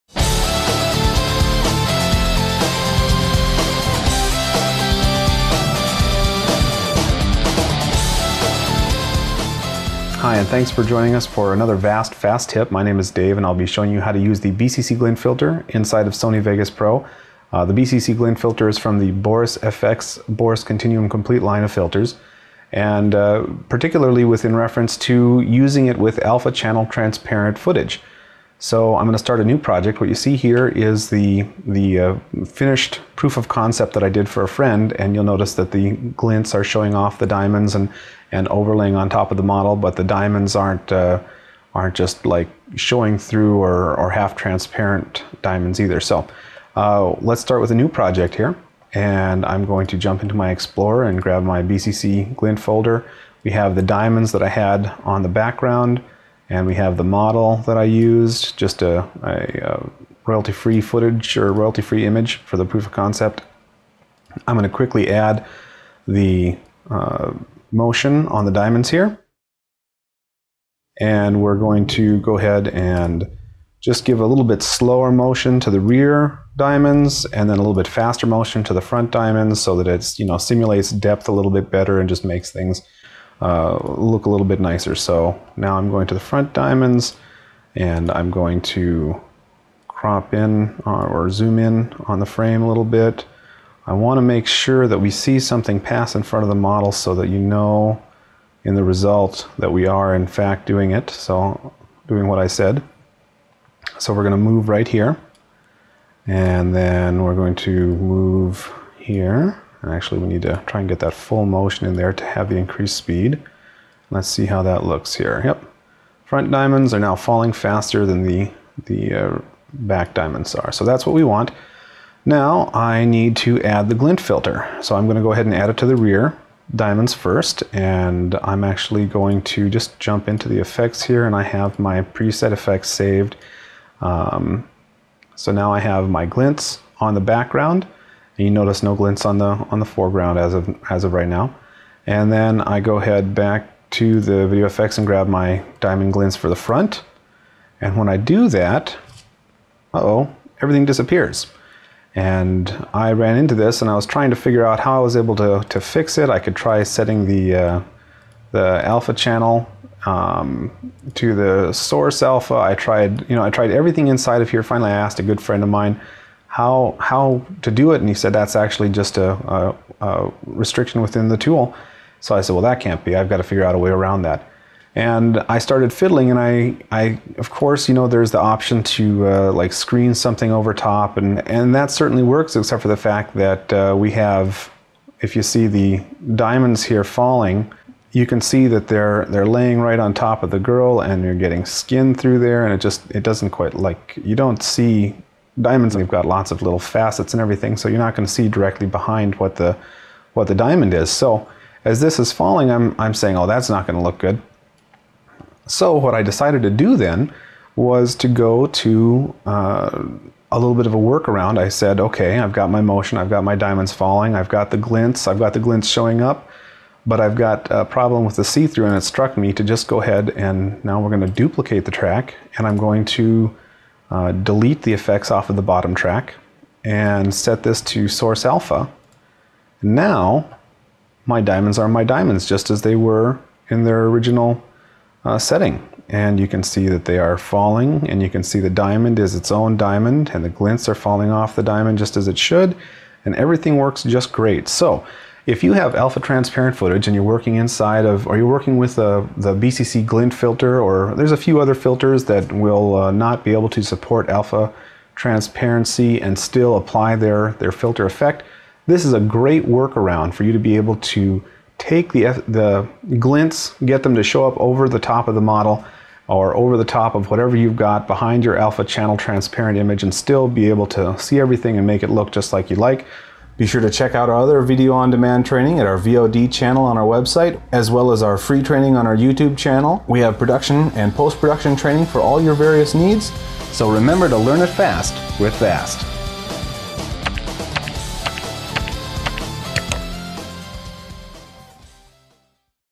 Boris FX Boris Continuum Complete Glint filter over alpha transparency quick tip
This short video is just a quick tip demonstrating a workflow to allow the use of filters like the Boris FX BCC Glint filter on alpha transparent footage while retaining the ability to overlay the footage, and the glints on other content below.  This is demonstrated in Sony Vegas Pro 10, however it's useful to demonstrate the workflow for any tool capable of using the BCC plug-ins from BorisFX.